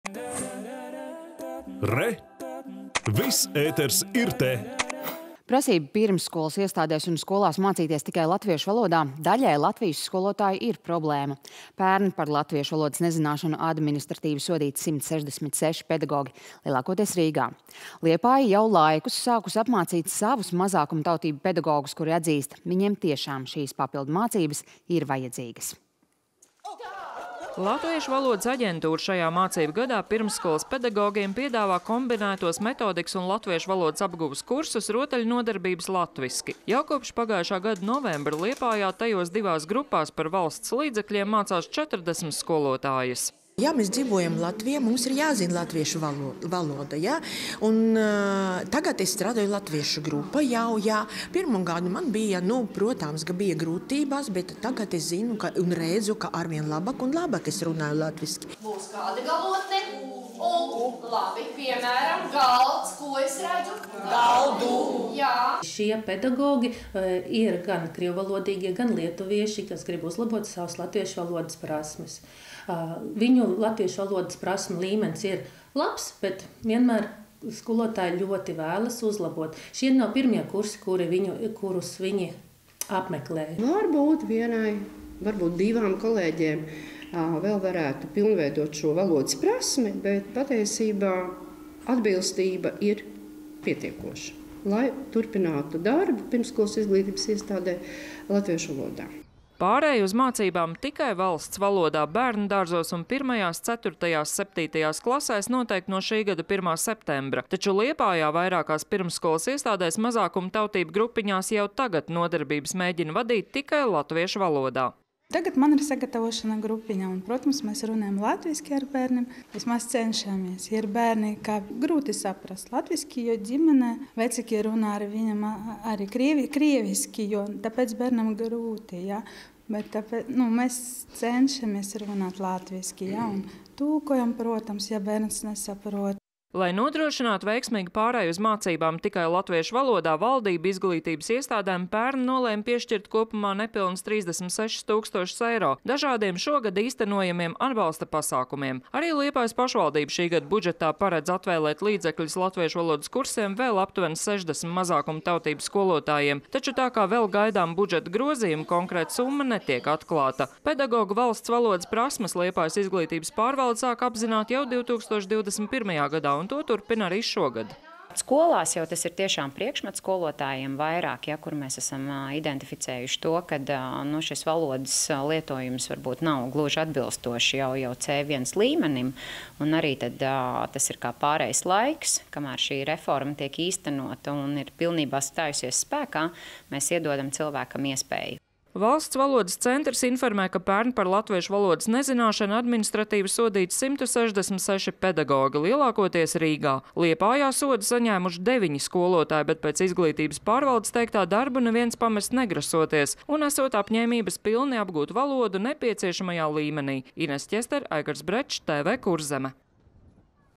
Liepājā mazākumtautību pedagogi mācās latviešu valodu
No šā gada 1. septembra bērni visās pirmsskolas iestādēs un vairākās pamatskolu klasēs mācīsies tikai latviešu valodā. Šī prasība daļai Latvijas skolotāju ir problēma, un pērn par latviešu valodas nezināšanu administratīvi sodīti 166 pedagogi – lielākoties Rīgā. Liepāja jau laikus sākusi apmācīt savus mazākumtautību pedagogus, kuri atzīst – viņiem tiešām šīs papildu mācības ir vajadzīgas.